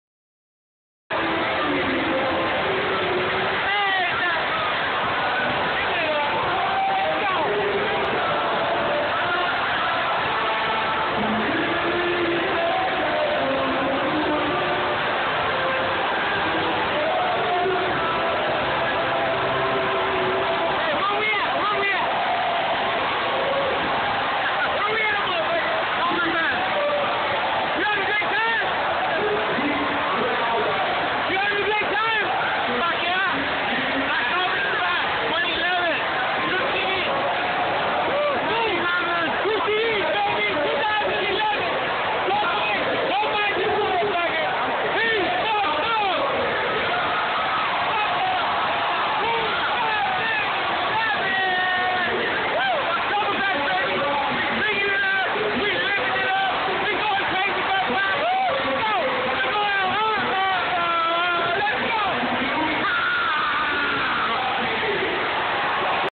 Oktoberfest 2011 in Torrance Ca (Alpine Village)
Torrance, Ca (Alpine Village) OKTOBER FEST 2011 October 14th 2011. De La Cruz interviews his friends and makes sure they are partying the right way.